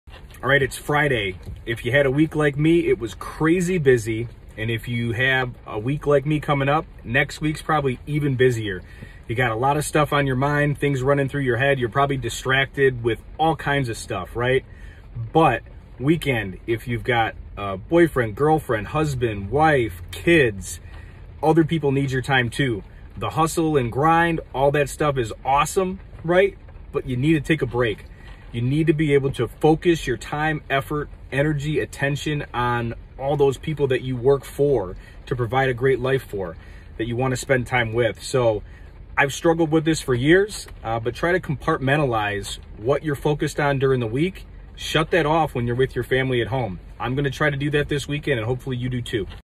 Compartmentalize - Shut It Off
If you’re crazy busy like me then you know it’s hard to shut it off. Try to focus on compartmentalizing your life to reduce stress and put your attention in what’s most important.